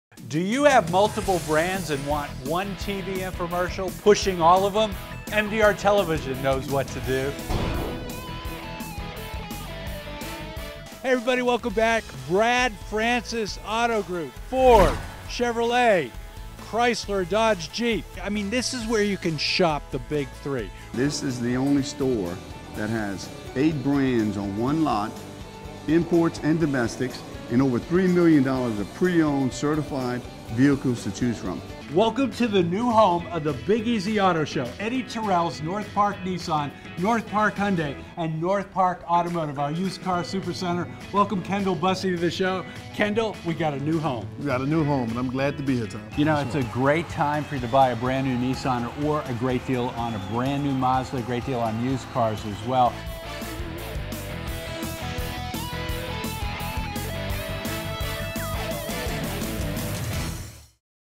Automotive TV Infomercials - Dealership Groups - Multi Brand Infomercials
MDR Television Inc., produces on location TV infomercials for dealership groups. Push multiple brand advertising in a 30 Minute TV format.

Shake up the advertising at your dealership group by producing 1 TV Infomercial usable for all your new car brands.

Saturday and Sunday mornings and afternoon airtime slots on your local ABC, NBC, CBS, and FOX tv stations where lots of eye balls can be found.

Take live phone calls during the show airing on Saturdays. Blow up your BDC.